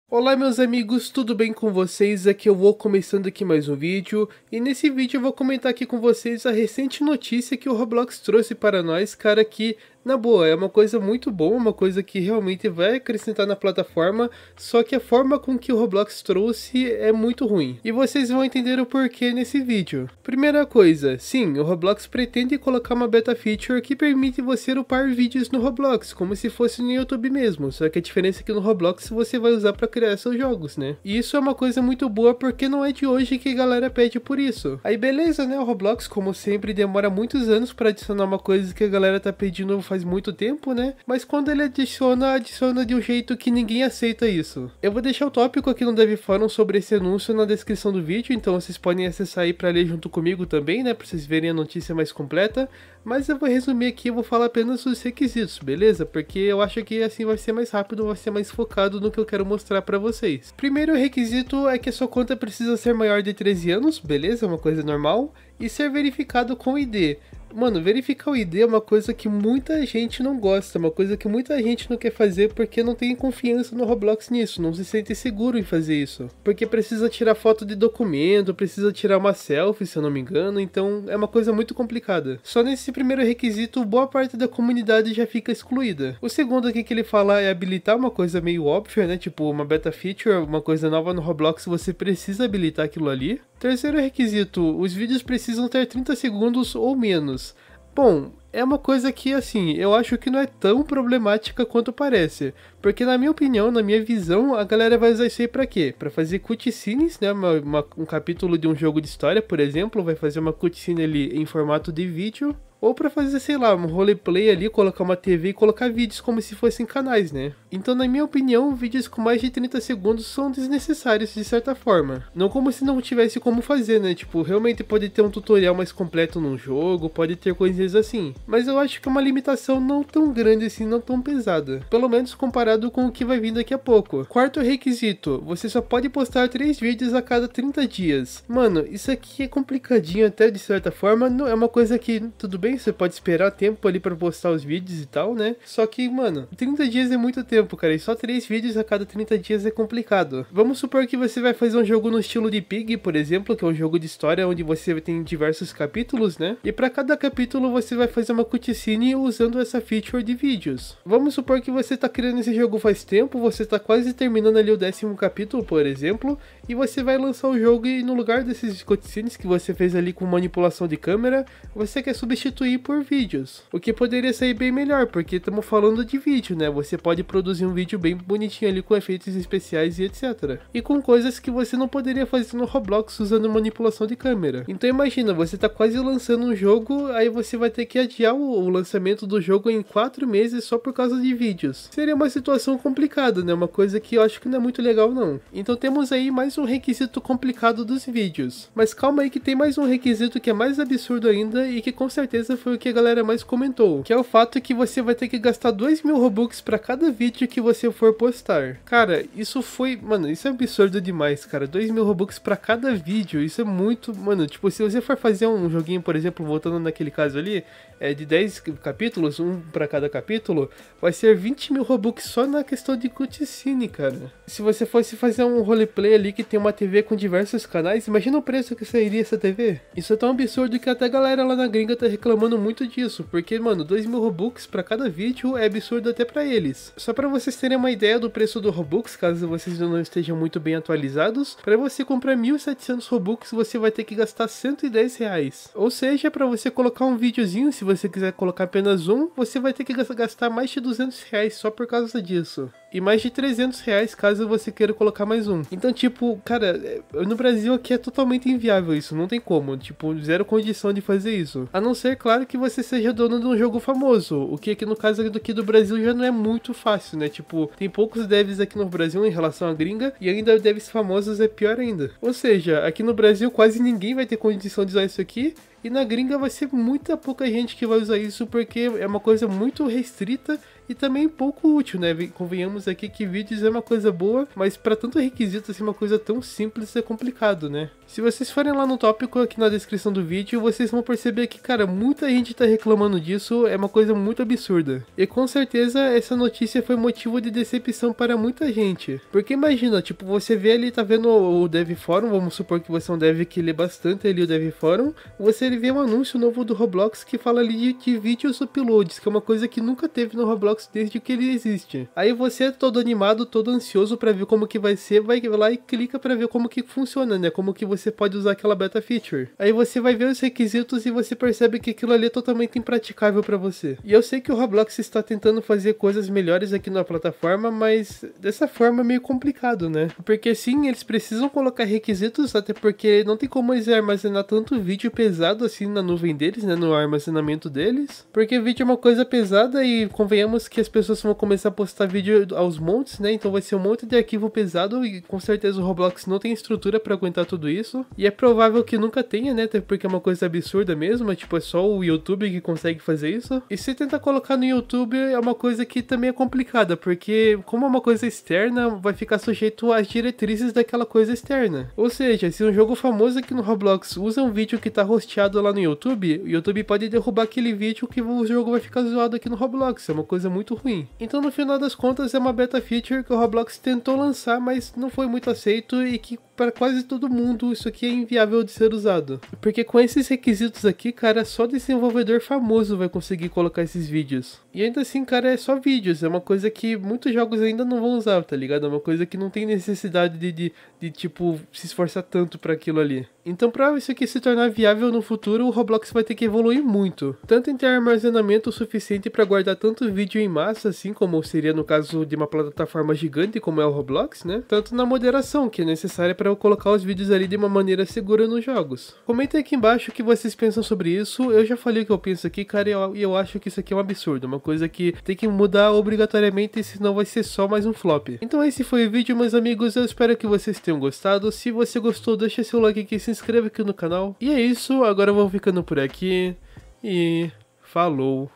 ROBLOX e o "Video Uploads"...
Nesse vídeo eu comentei sobre o recente anúncio do Roblox sobre uma beta feature chamada "Video Uploads", e sobre a decepção que isso foi para os devs...

(*) SOCIAL (*)